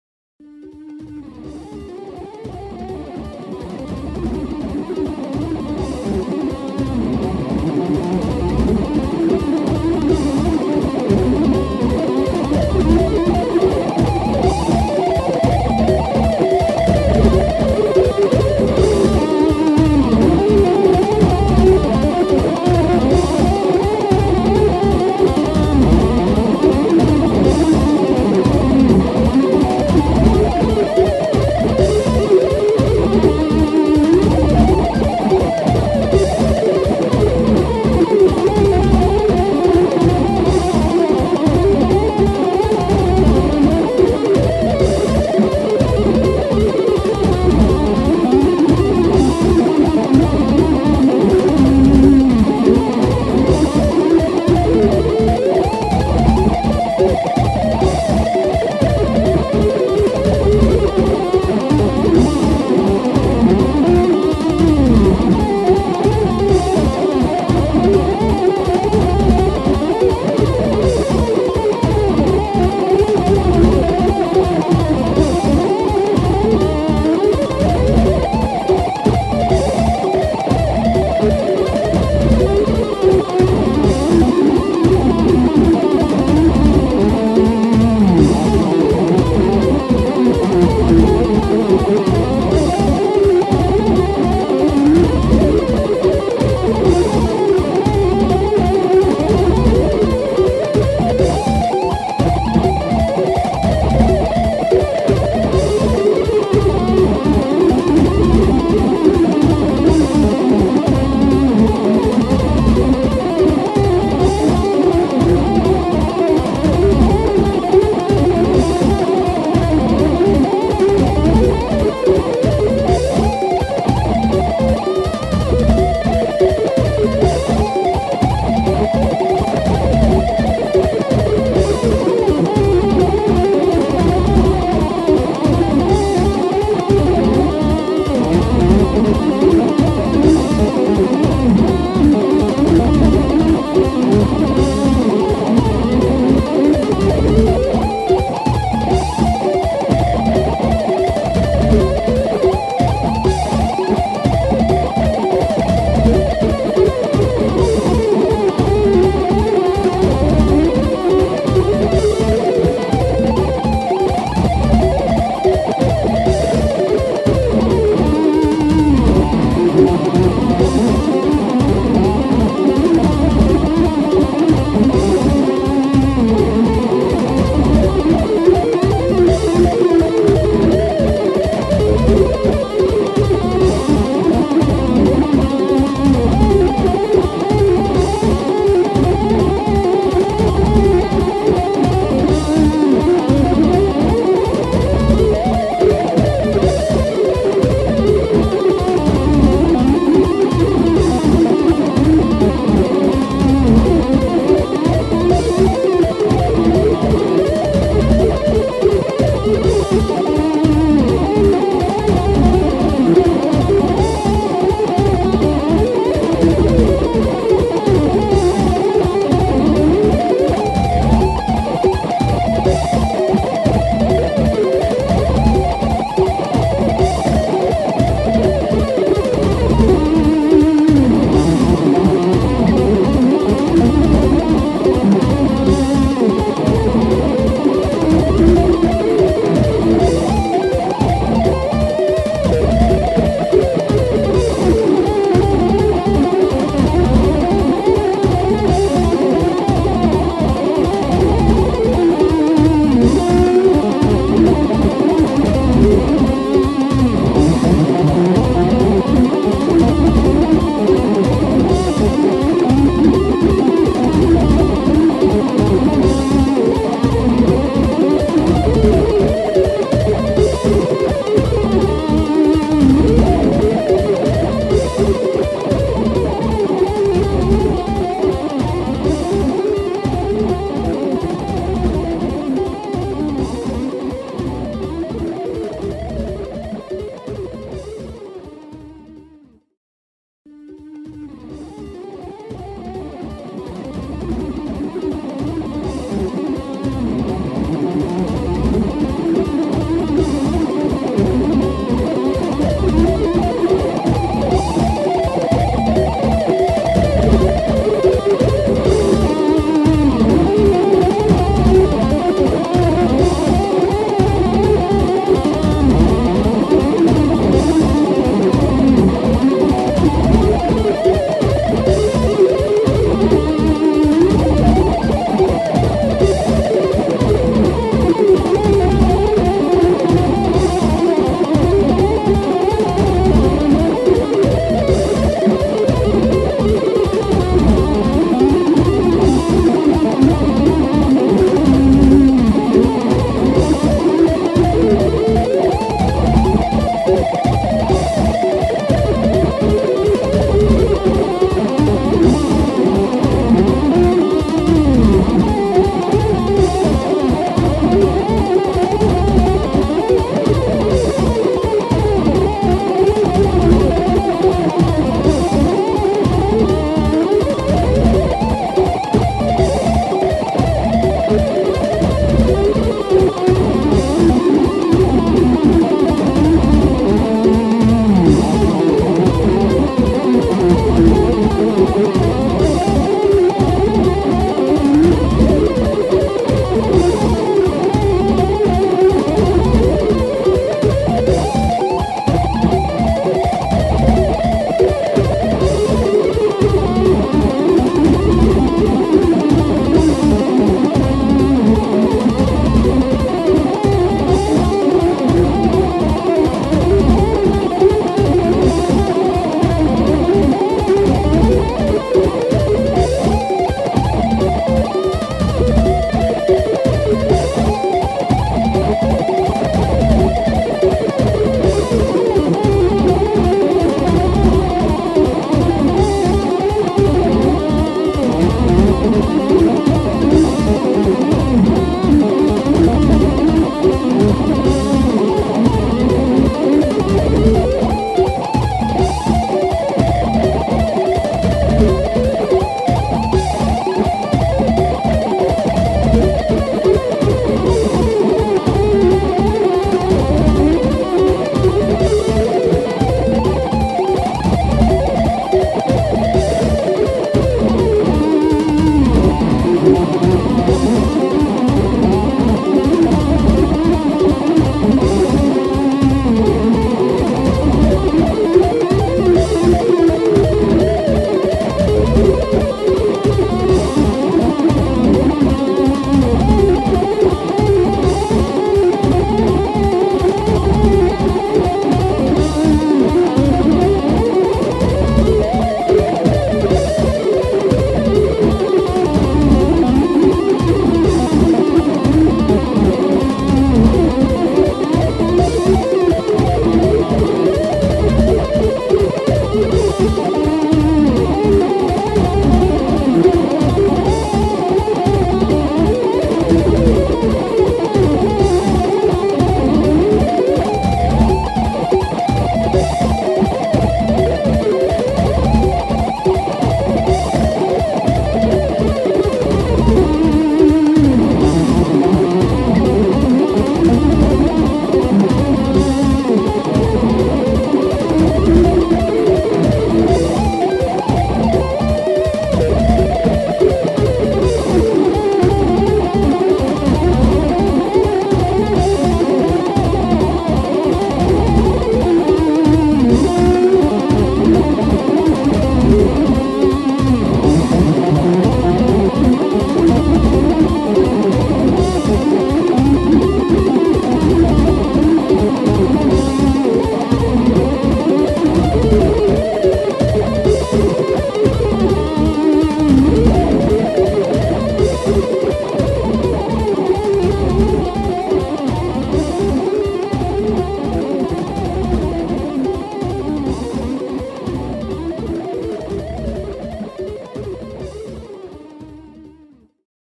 Latin freestyle fingerstyle fusion rock guitar solo
Live fingerstyle guitar solo     single take  unedited session  2022